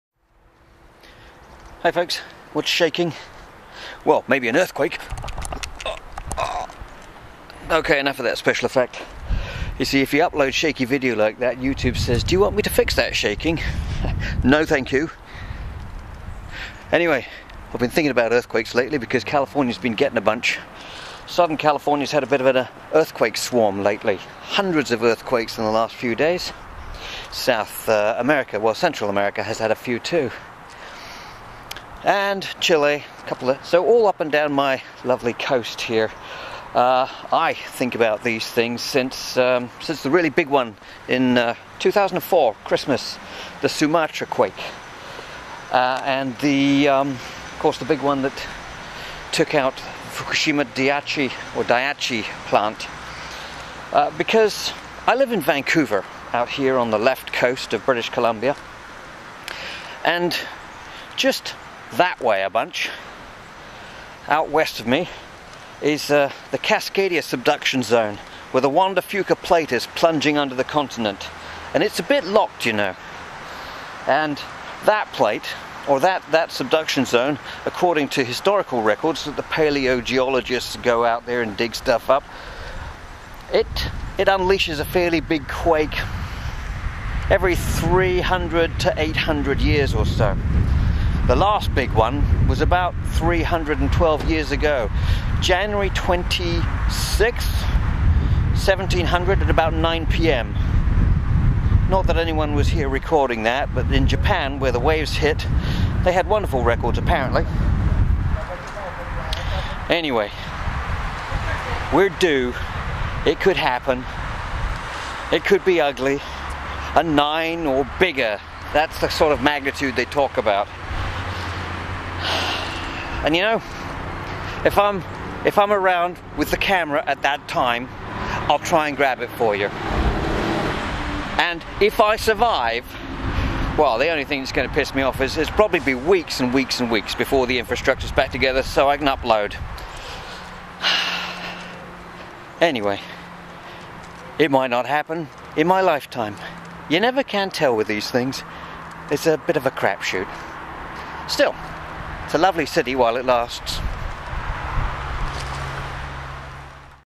One Day the Ground will Shake.
It's innevitable in the end. It's just a fact of life (and death) where I live. You do what you can to prepare but we've all got to go at some point and you can't prepare for everything.

This blurb is just something that I was thinking about in the last couple of days. I'm not worried, but when you hear about shaking nearby it reminds you of what's under your own feet.